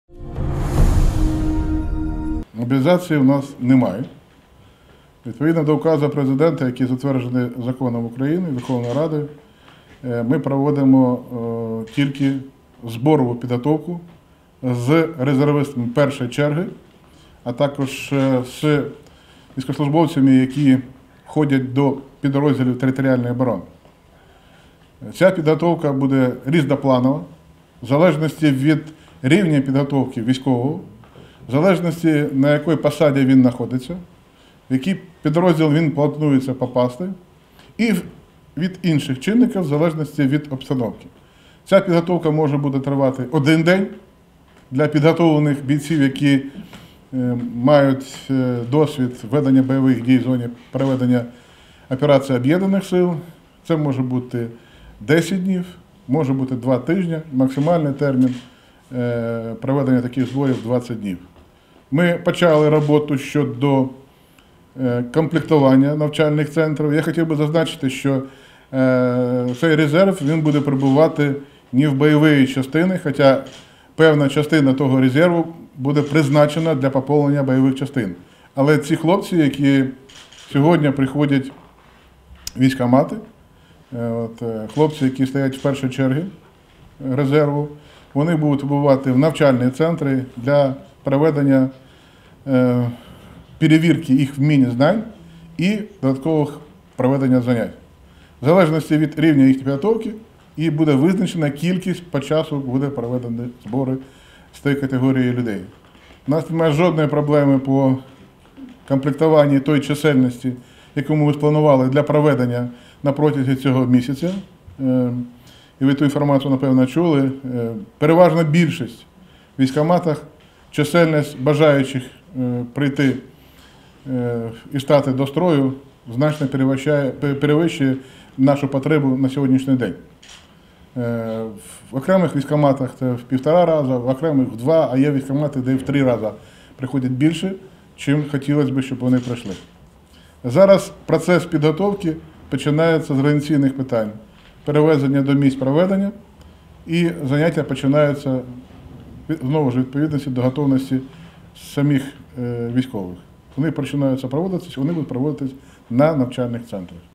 "МОБІЛІЗАЦІЇ У НАС НЕМАЄ"
«Мобілізації у нас немає. Ми проводимо лише збори і підготовку з резервістами оперативного резерву першої черги, а також з військовослужбовцями, які входять до підрозділів територіальної оборони». На цьому сьогодні, 5 грудня, під час брифінгу для представників ЗМІ наголосив Міністр оборони України Степан Полторак.

Він підкреслив, що ця підготовка різнопланова і залежить від рівня підготовки військовослужбовця, на якій посаді він знаходиться, в які підрозділи він планується до розподілу, а також від інших чинників залежно від обстановки.

— Ця підготовка триватиме як один день для військових, які мають досвід участі у бойових діях. Також це може бути 10 днів, може бути і два тижні. Максимальний термін проведення — 20 днів, — повідомив Степан Полторак.

Глава оборонного відомства поінформував про початок роботи щодо комплектування навчальних центрів Збройних Сил України.

— Цей резерв перебуватиме не в бойових частинах, хоча певна частина того резерву буде призначена для поповнення бойових частин. Але ці хлопці, які сьогодні приходять у військкомати і стоять в першій черзі резерву, будуть відбувати в навчальні центри для проведення перевірки їх вмінь, знань і проведення занять. Зараз процес підготовки починається з організаційних питань перевезення до місць проведення, й заняття починаються відповідно до підготовки самих військових, — розповів він.

— Жодних проблем з комплектуванням немає. Переважна більшість бажає прийти до військкоматів і стати до строю. Це значно перевищує нашу потребу на сьогодні, — підкреслив Міністр оборони України Степан Полторак.